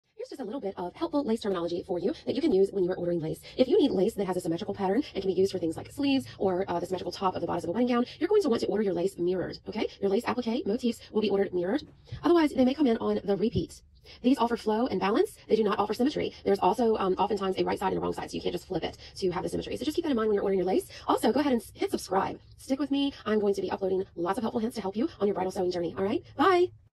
Helpful terminology for when you are ordering lace | bridal lace fabric appliqués motifs
Helpful terminology for when you are ordering lace appliqués or motifs.

I help sewists break into the bridal sewing industry.